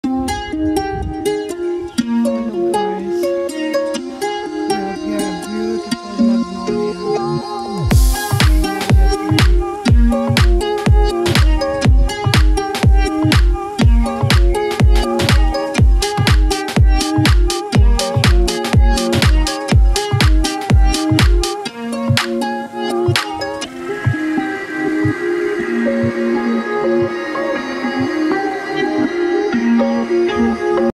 Fascinating Magnolia Tree in full Bloom Spring Seasons in Germany
Fascinating Magnolia Tree in full Bloom in Spring Seasons Germany
In Germany we have Four Seasons in a year and  SpringSeasons is one of  my favorite among of them, a seasons of beautiful different Flowers everywhere and mostly mild sunny weather thats the reason why i love Springtime.